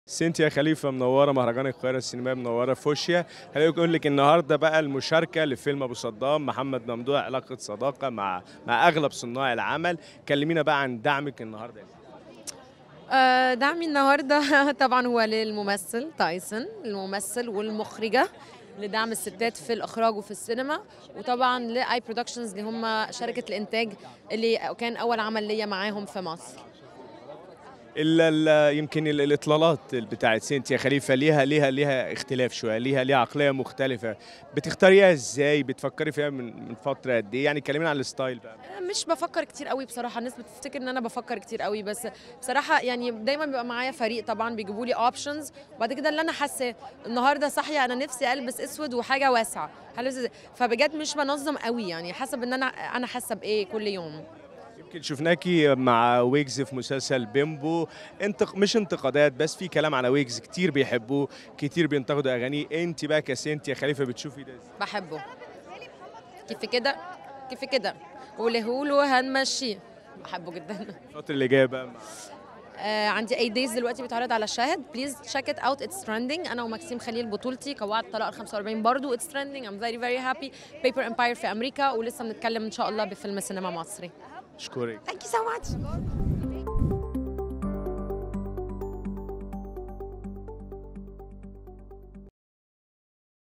سنتيا خليفة : أحب ويجز.. وهكذا اختار إطلالاتي
أكدت الفنانة سنتيا خليفة أنها تقوم باختيار إطلالاتها بشكل مختلف، ولا تفكر تماما في هذا الأمر، بل تختار حسب مزاجها في نفس اليوم.